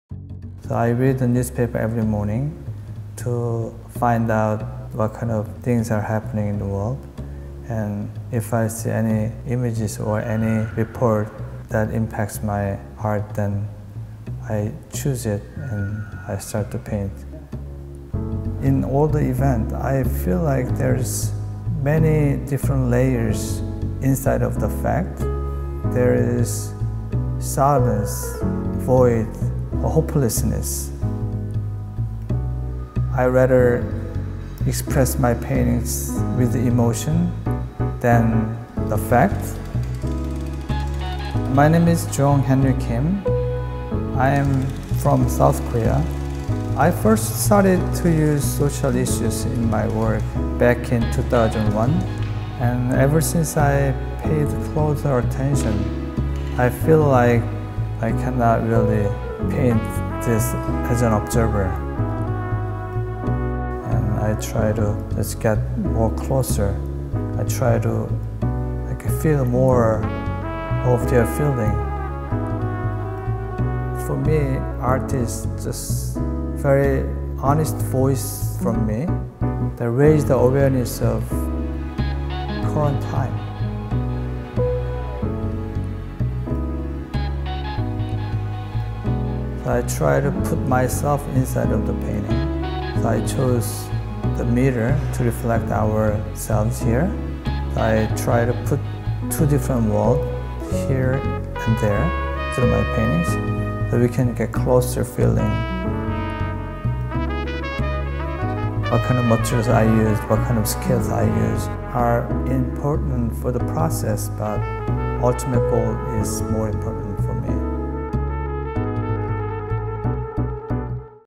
Artist Henry Kim'S Interview
Artist Henry Kim captures the moments of time in the lives of people facing injustice, hate and violence around the world today.